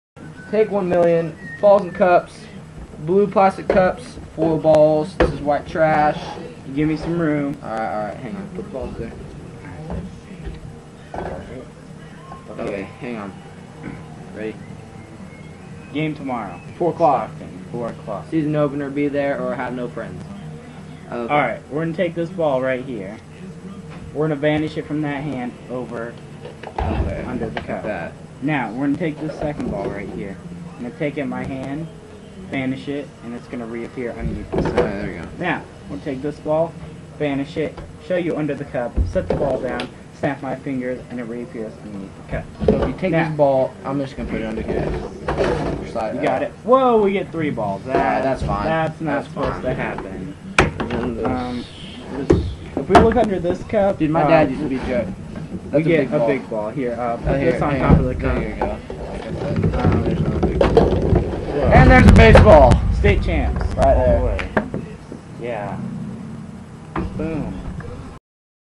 Cups and Balls with Mr. Magicman Gunnels
Cups and Balls, a twist on the Penn and Teller version, we plugged our baseball game for facebook :)